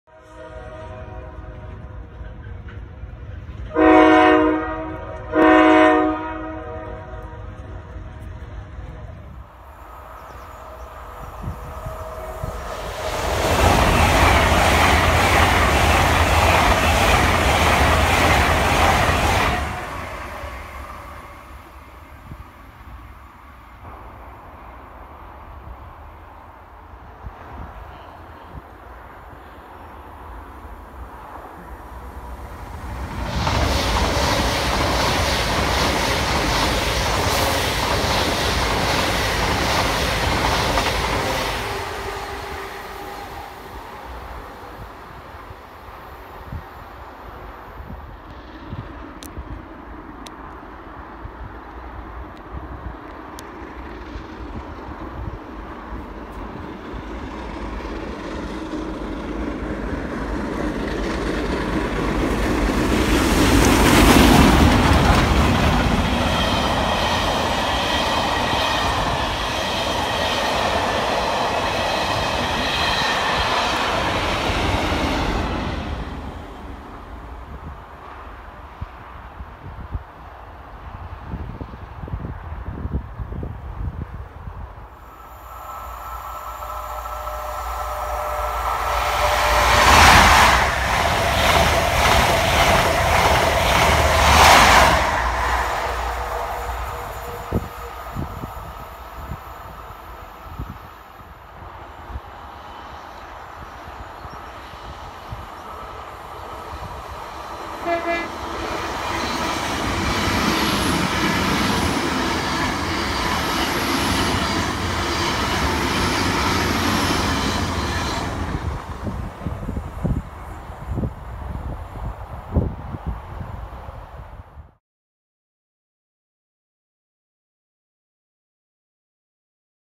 Trains at Tiverton Parkway. FT 46100 Royal Scot and 45231 The Sherwood Forester
In this video you will be seeing services from GWR,CrossCountry and Saphos Trains

The Saphos Trains locos were 46100 Royal Scot and 45231 The Sherwood Forester